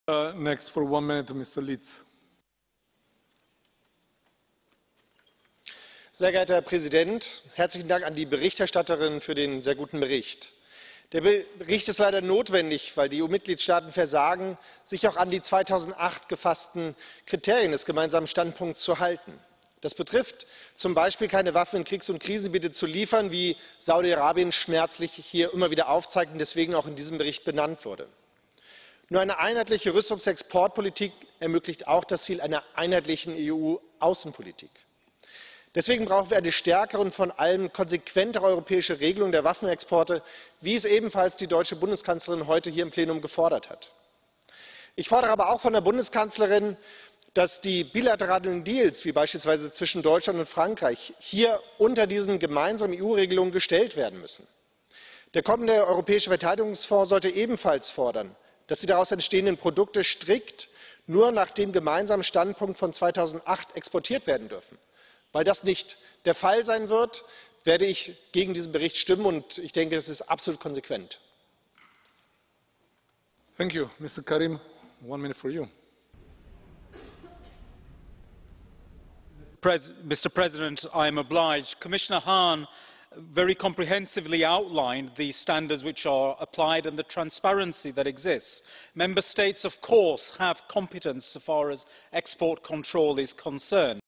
Arne Lietz - Rede im Europäischen Parlament: Waffenexporte (13.11.2018)
Arne Lietz Plenarrede zu Waffenexporten am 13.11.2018